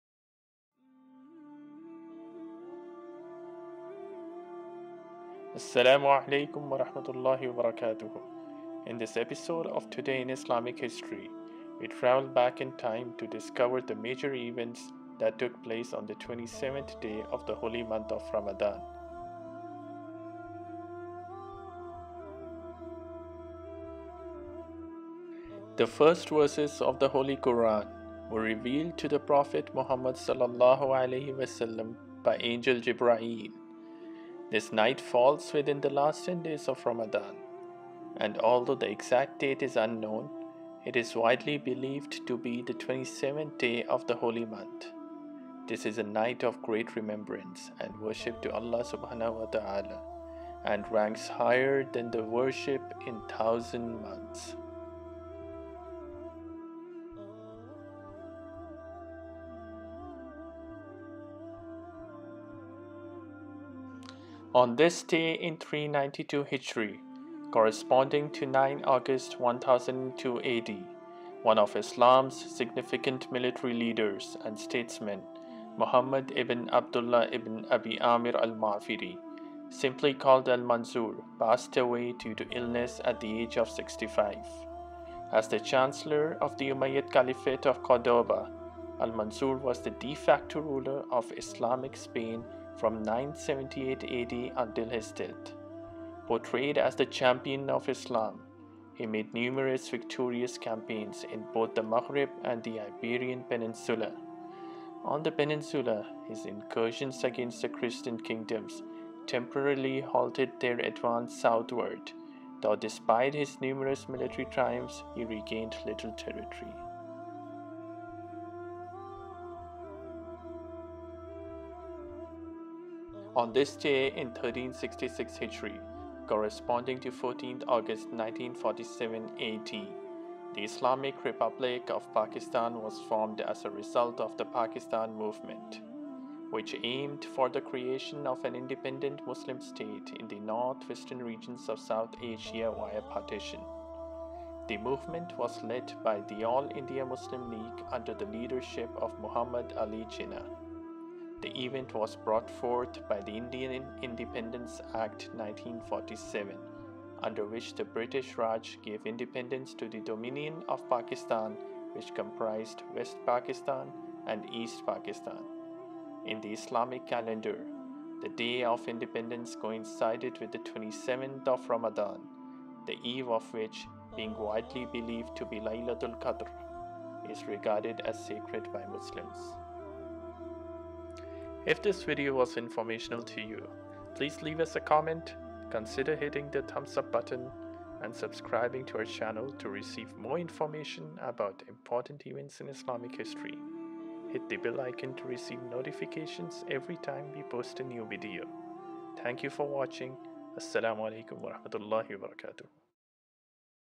Important Islamic History Events - 27th Ramadan Events | Today in Islamic History
This video describes the significant events that happened on 27th Ramadan. Some of the most important Islamic History events occurred during the month of Ramadan.

Significant events covered in this video:
Revelation of the Holy Qur'an - The first verses of the Holy Qur’an were revealed to the Prophet Muhammad (SAW), by Angel Jibrail. This night falls within the last 10 days of Ramadan, and although the exact date is unknown, it is widely believed to be the 27th day of the Holy month. This is a night of great remembrance and worship to Allah (SWT) and ranks higher than that of worship in 1,000 months.

Death of Almanzor - On this day in 392 Hijri, corresponding to 9 August 1002 AD, one of Islam’s significant military leaders and statesmen, Muḥammad ibn ʿAbdullāh ibn Abi ʿĀmir al-Maʿafiri (simply called Almanzor) passed away due to illness at the age of 65. As the chancellor of the Umayyad Caliphate of Córdoba, Almanzor was the de facto ruler of Islamic Spain from 978 AD till his death. Portrayed as the champion of Islam, he made numerous victorious campaigns in both the Maghreb and the Iberian Peninsula.[14] On the peninsula, his incursions against the Christian kingdoms, temporarily halted their advance southward, though despite his numerous military triumphs he regained little territory.
References:
For information on Almanzor - The Tibyān: Memoirs of ʻAbd Allāh B. Buluggīn, Last Zīrid Amīr of Granada

Independence of Islamic Republic of Pakistan - On this day in 1366 Hijri, corresponding to 14th August 1947 AD, the Islamic Republic of Pakistan was formed as a result of the Pakistan Movement, which aimed for the creation of an independent Muslim state in the north-western regions of South Asia via Partition. The movement was led by the All-India Muslim League under the leadership of Muhammad Ali Jinnah. The event was brought forth by the Indian Independence Act 1947 under which the British Raj gave independence to the Dominion of Pakistan which comprised West Pakistan (present-day Pakistan) and East Pakistan (now Bangladesh). In the Islamic calendar, the day of independence coincided with the 27th of Ramadan, the eve of which, being widely believed to be Laylat al-Qadr, is regarded as sacred by Muslims.